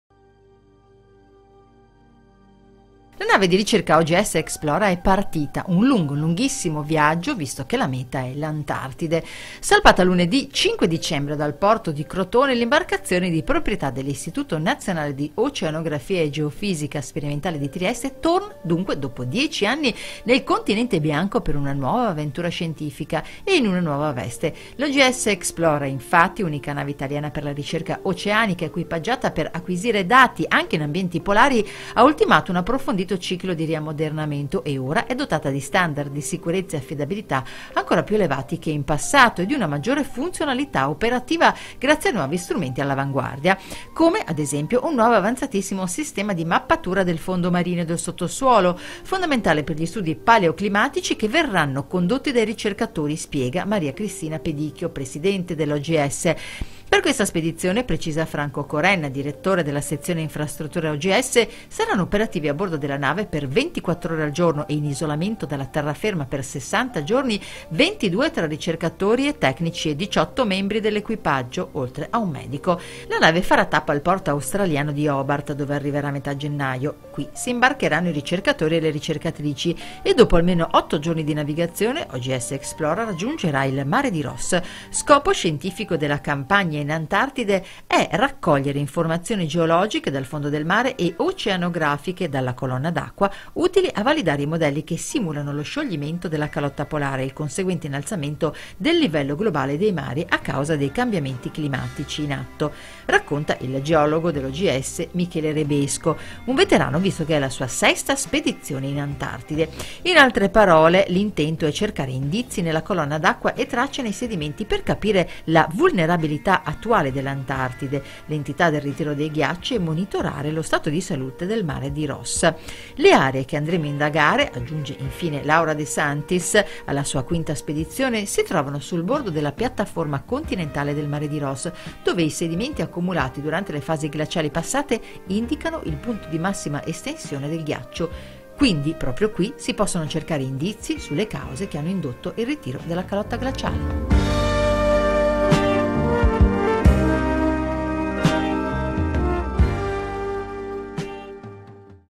DIREZIONE ANTARTIDE
Un gruppo di scienziati dell’Istituto Nazionale di Oceanografia di Trieste in viaggio - bordo di una nave speciale -  per studiare i cambiamenti climatici